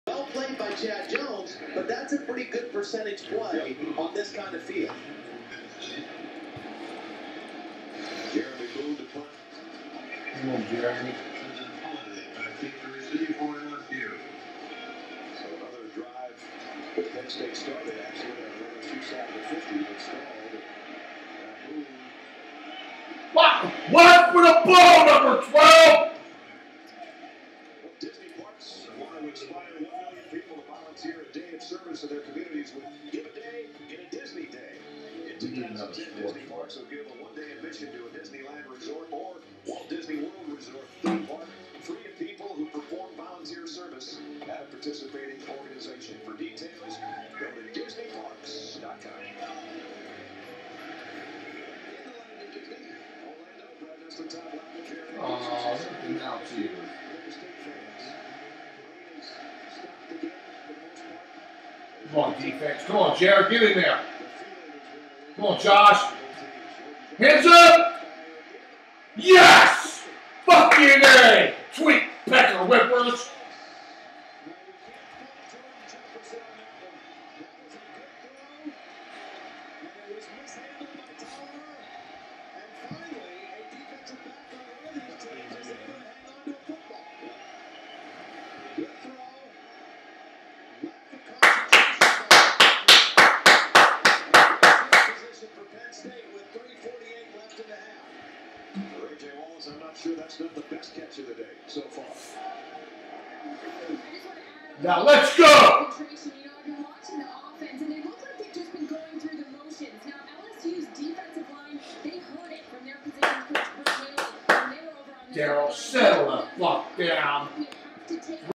Pecker Whiffer!!!
Took this video its my dad watching the penn state, rose bowl game. They score a touch down and he's say's the funniest thing ever!!